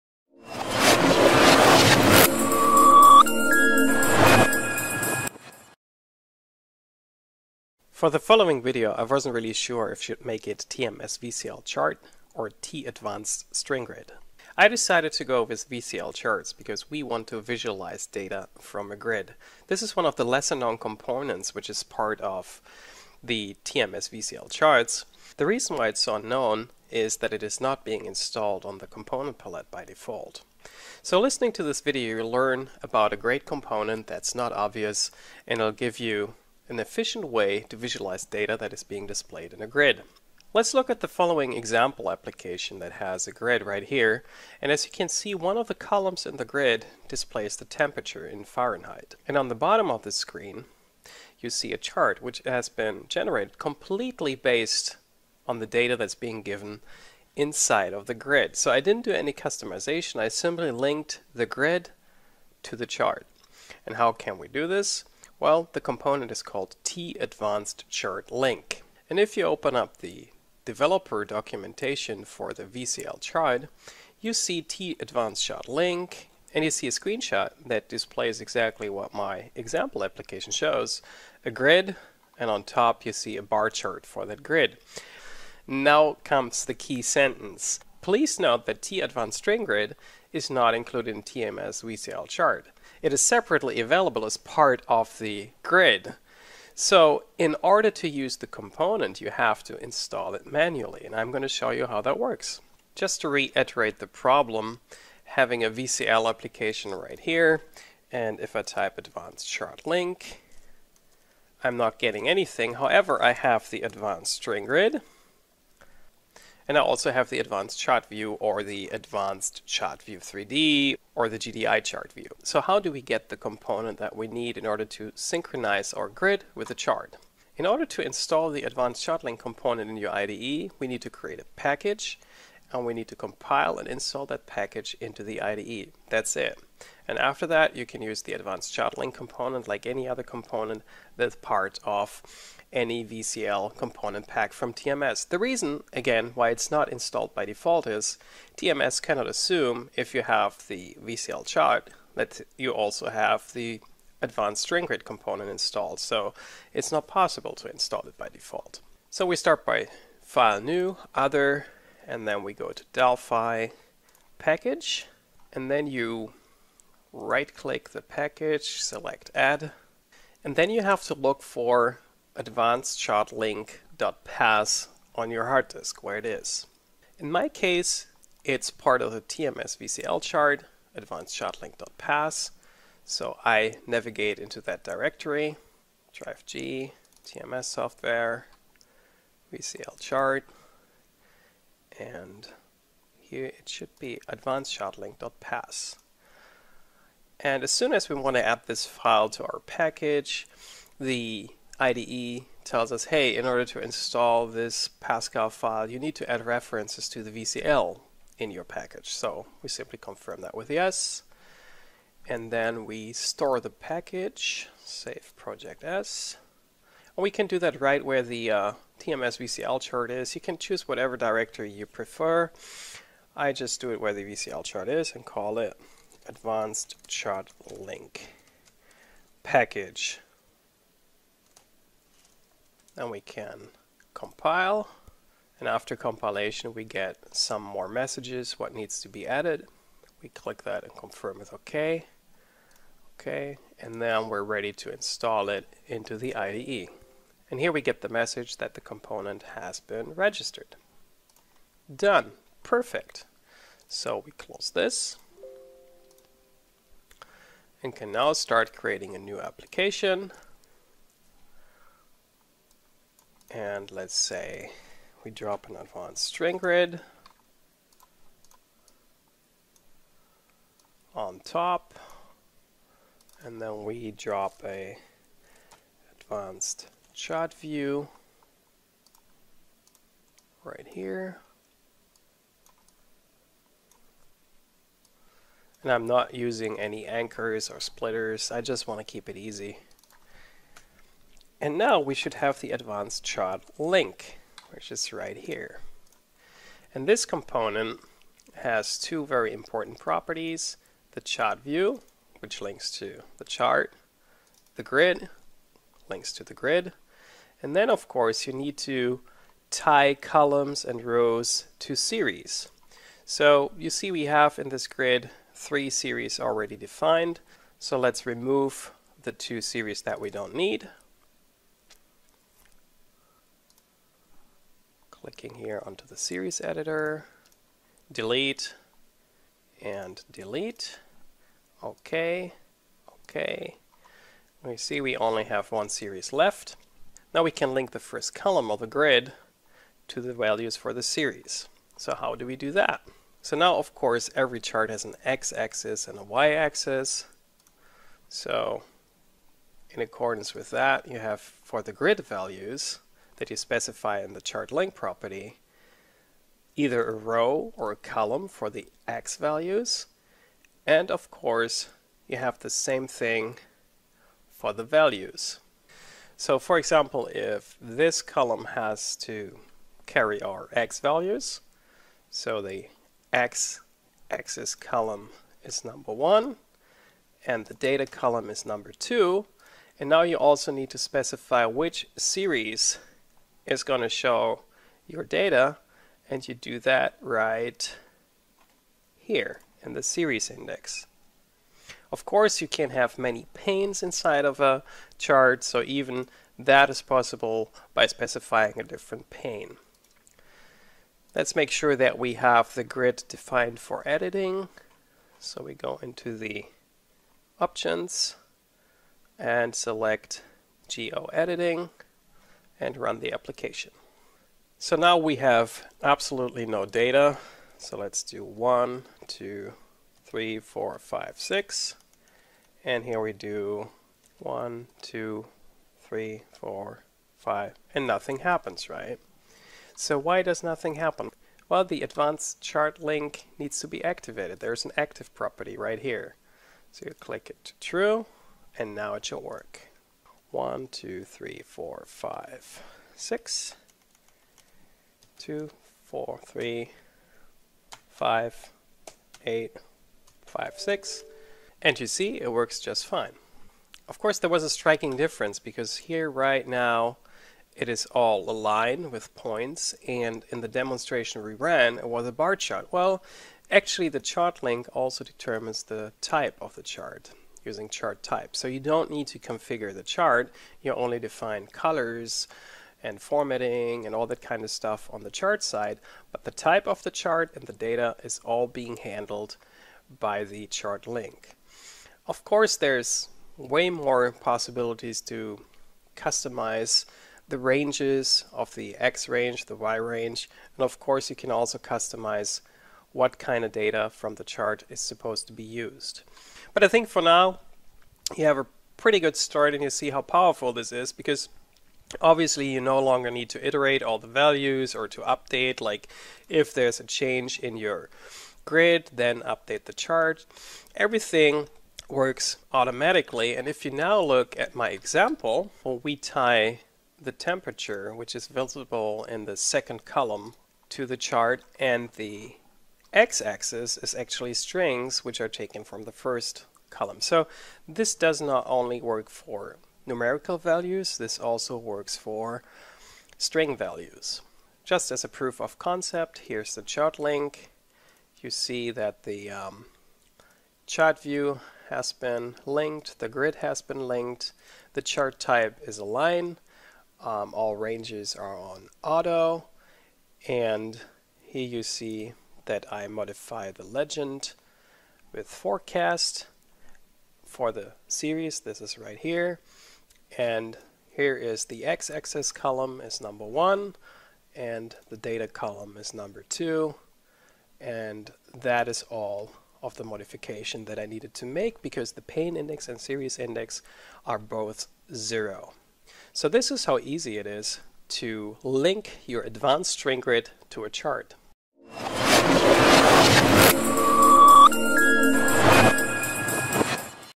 TMS VCL Charts: Efficiently visualizing data from a grid
Learn how to seamlessly integrate TMS VCL Chart with TAdvanced String Grid in our latest tutorial video. Learn how to effortlessly visualize grid data in dynamic charts, uncovering hidden potential in your applications. Discover the step-by-step process of manual component installation and synchronization for efficient data representation. Dive into the power of AdvancedStringLink and ChartView, transforming your data visualization experience.

ABOUT:
TMS software, established in 1995, is a software development company specialized in: